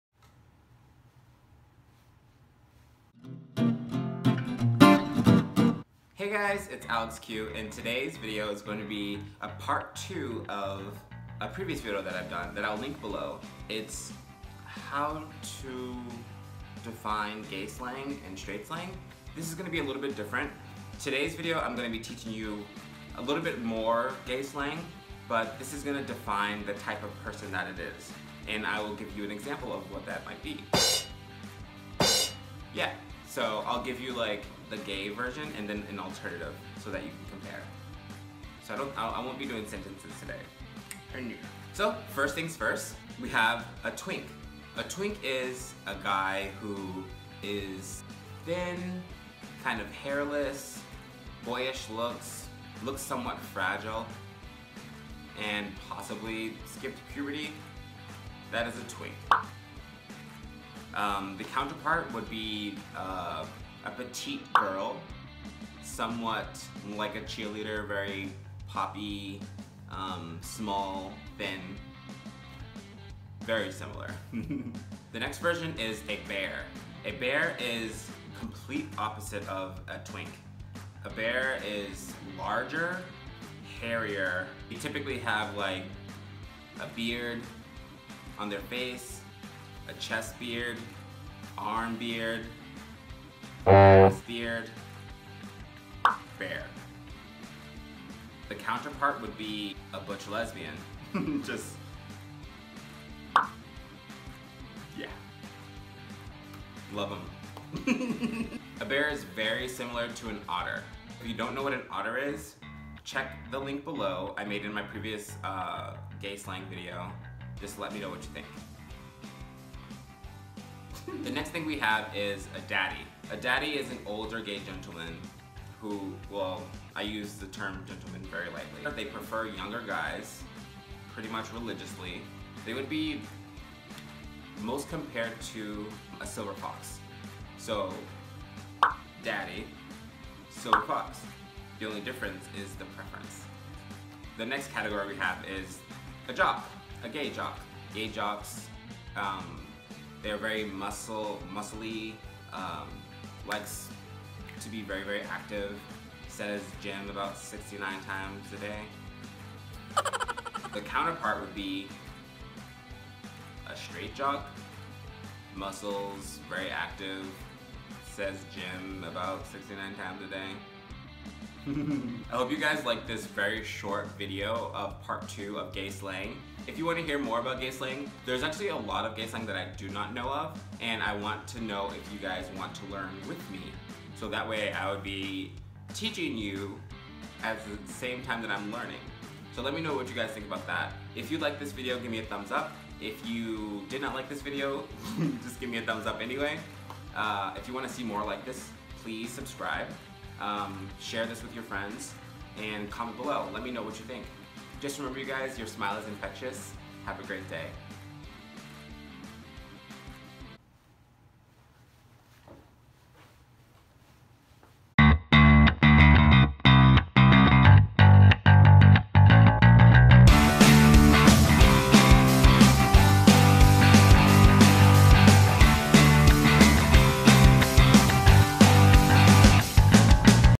Learn Gay Slang (Part 2)
Still trying to figure out Gay Lingo? Well I am here to lend a hand. Please enjoy part 2 of me teaching you gay slang. This time we are learning about how some guys identify or even label one another based on looks. :)

Please Like/Share/Comment if you enjoyed the video or if you think my random silliness can bring a smile to someone.